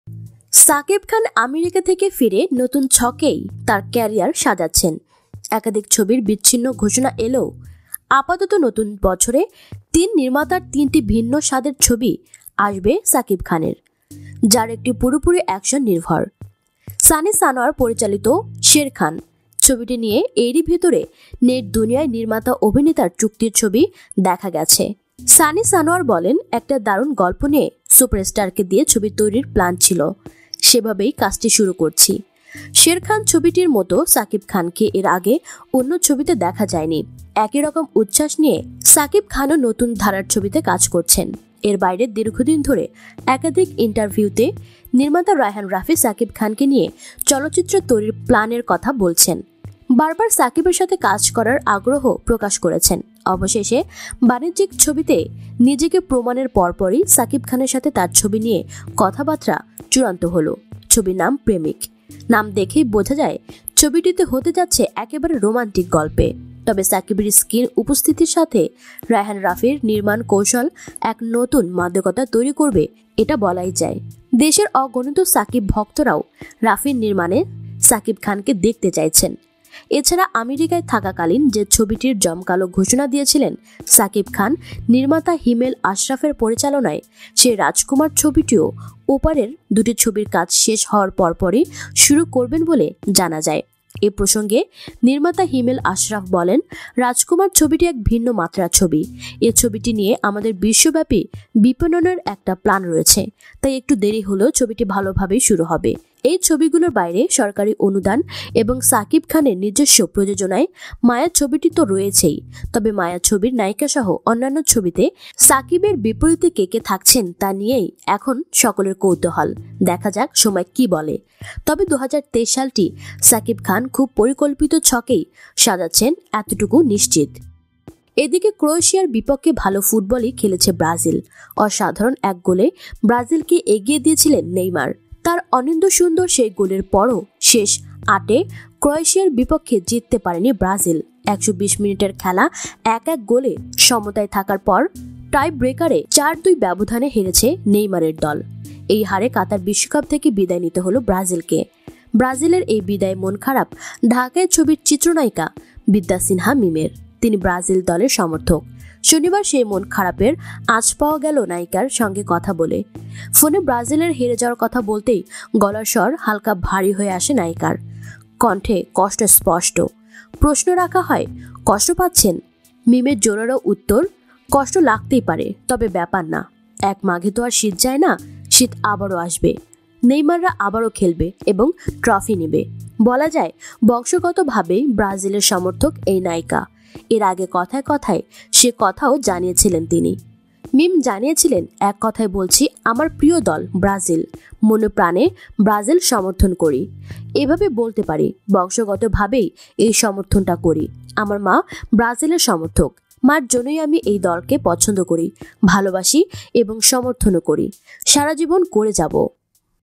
২০২৩ সাল শাকিবের পরিকল্পিত , সেরা সিনেমা আসবে এ বছর | Sherkhan | Premik | Rajkumar | Maya | Agun
শাকিব খান আমেরিকা থেকে ফিরে নতুন ছকেই তার ক্যারিয়ার সাজাচ্ছেন। একাধিক ছবির বিচ্ছিন্ন ঘোষণা এলেও আপাতত নতুন বছরে তিন নির্মাতার ৩টি ভিন্ন স্বাদের ছবি আসবে শাকিব খানের। যার একটি পুরোপুরি অ্যাকশন নির্ভর। সানী সানোয়ার পরিচালিত ‘শের খান’ । ছবিটি নিয়ে এরই ভেতরে নেটদুনিয়ায় নির্মাতা-অভিনেতার চুক্তির ছবি দেখা গেছে।

শীর্ষের সেরা ১০ টি জম্বি মুভি যা বক্স অফিসে এখন পর্যন্ত সবার উপরে । দেখে নিন কোন মুভি গুলো